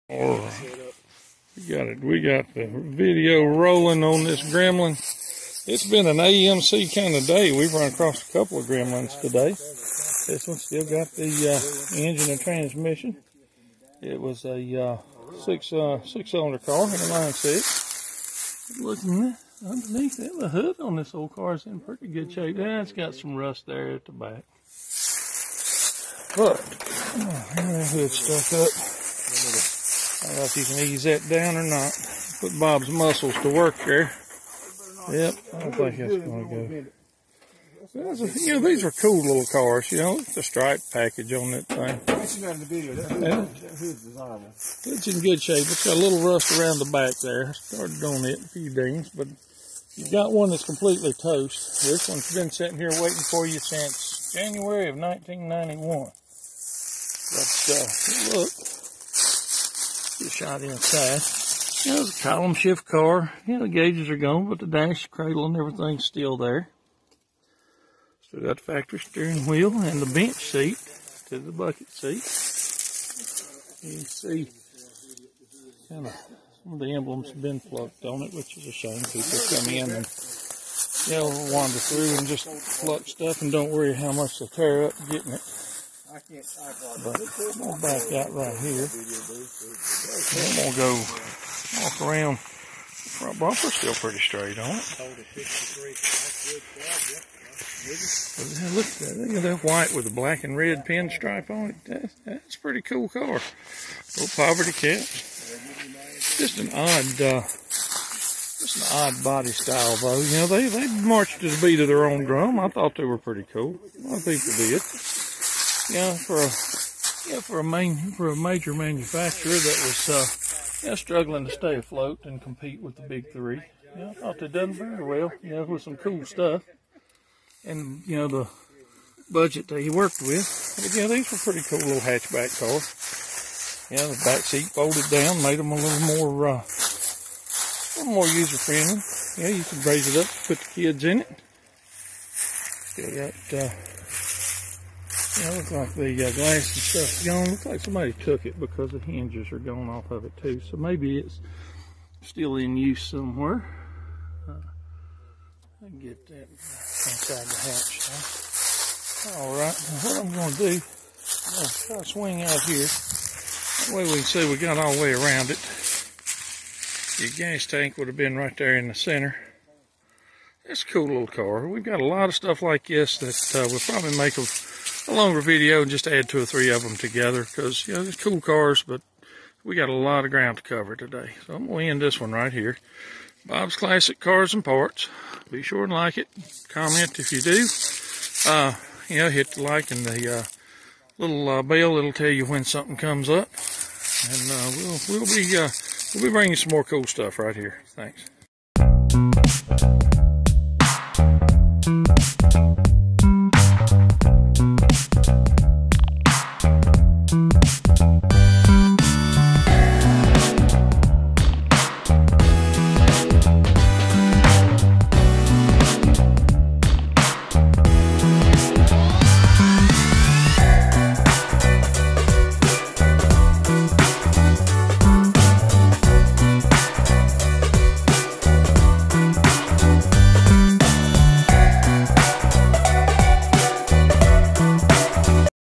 Found another Gremlin in an old junkyard
The dickens you say...not another Gremlin. Yes, yet another one sitting in a heavily wooded old yard up on a hill near a stream.